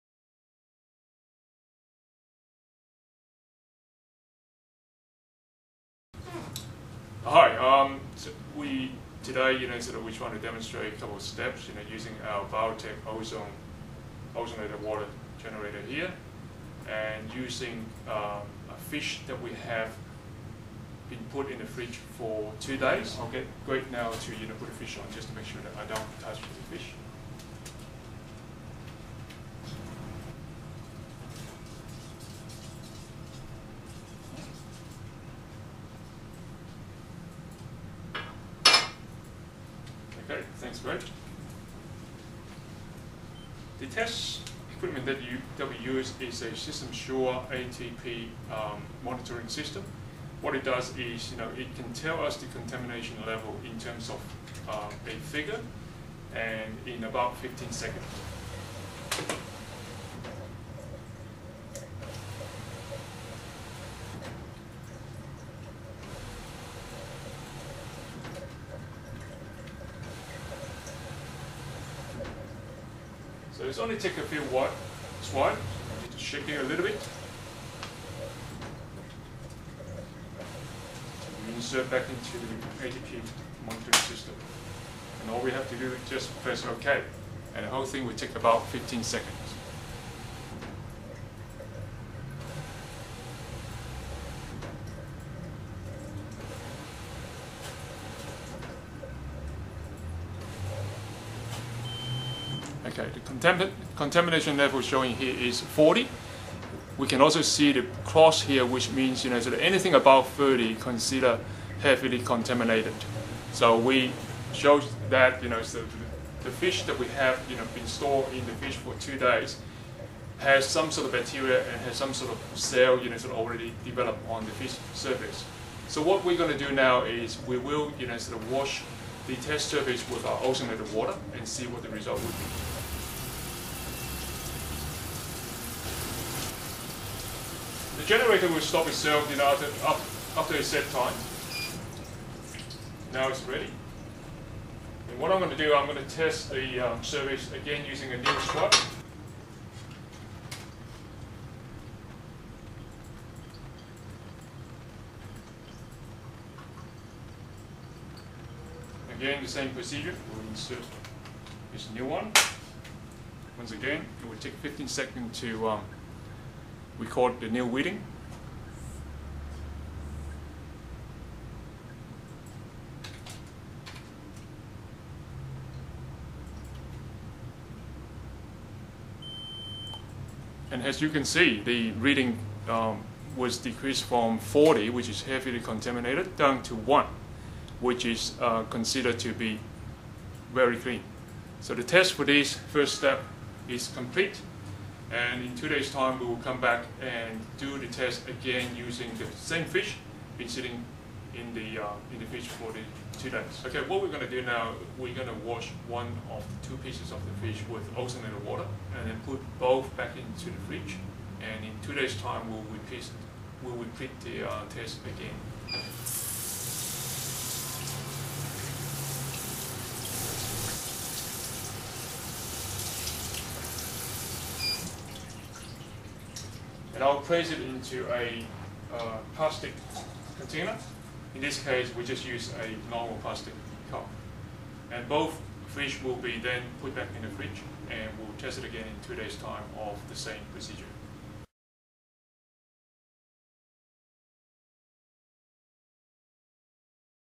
Ozonated water used on Fish bacteria after 2 days
Fresh fish tested for bacteria after being stored in fridge for 2 days. Test surface then cleaned with ozonated water and re-tested showing the effectivness of ozonated water in reducing bacteria.

Please ensure you watch the follow-up video entitled "Ozonated water used on Fish bacteria after 4 days"